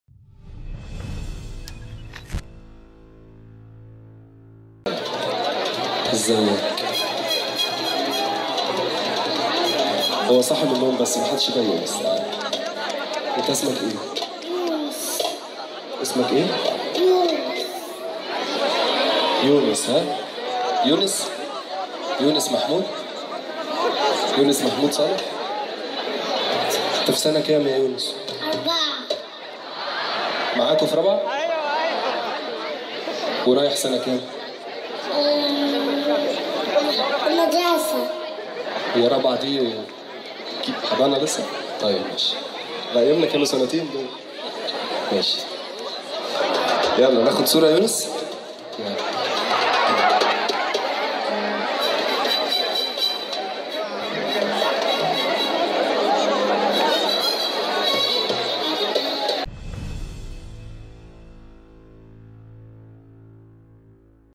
أحمد جمال يداعب طفل أراد التصوير معه فى حفل أنت أقوى من المخدرات
داعب النجم أحمد جمال طفل أراد التقاط صورة معه أثناء إحيائه احتفالية تدشين حملة "انت أقوى من المخدرات" داخل جامعة عين شمس.